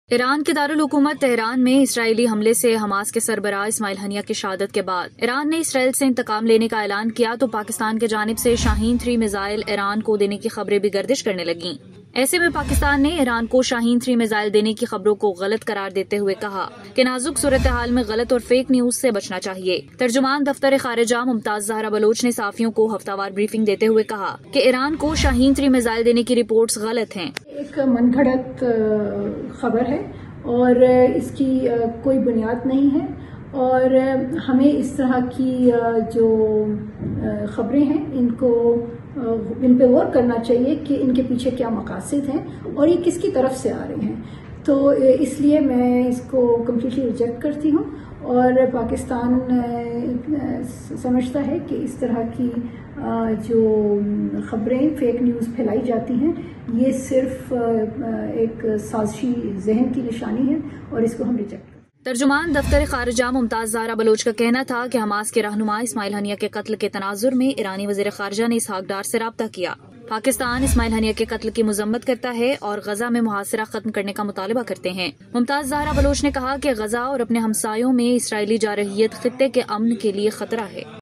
Pakistan Denies reports of Supplying Shaheen missiles to Iran | Rohi
Kya Iran ko Pakistan ny Israel Pr Hamly Ky Liye Shaheen 3 Missile Dia? | Rohi Special